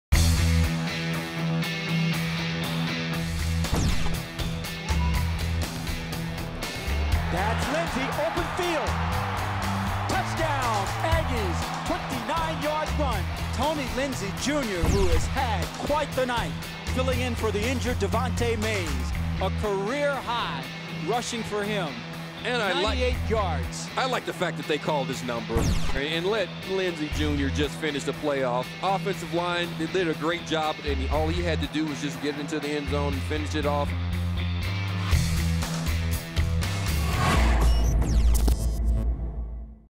Utah State's Tonny Lindsey Has Career Night | CampusInsiders
Utah State's Tonny Lindsey shined in the absence of the Aggie's senior running back Devante Mays against Arkansas State. Lindsey ran for a career-high 98 yards and two touchdowns to lead the Aggies to a 34-20 win over Arkansas State.